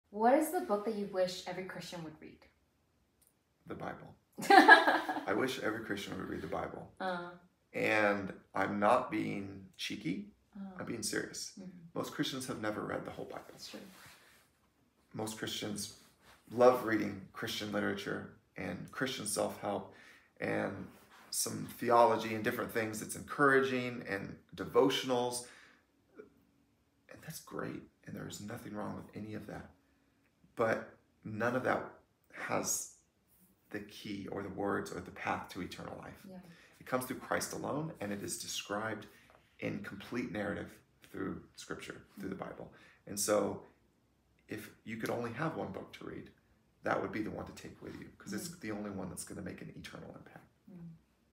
The book every Christian should read!
The Bible! Yes, it sounds obvious, right?

Other books can support our understanding, spirituality, and personal devotion, and spur us on in faith through encouragement, but only the Bible holds the complete counsel of God’s word and will for mankind.

No other book holds the words of eternal life (John 6:68), is the inspired word of God, is profitable for instruction in righteousness (2 Timothy 3:26–27), and is worth meditating on both day and night (Psalm 1:2).

The Bible covers the spectrum of human emotion, is honest with human frailty and sin, and provides a clear path for redemption and salvation through Jesus Christ.

Other books are beneficial, but, for the Christian, the Bible is the only book that is needful.

Christians ignore it at their own peril. Everyone who studies it and clings to its truth in faith does so for their own eternal well-being.

If you only have one book to study, I would highly recommend spending more time in the Bible.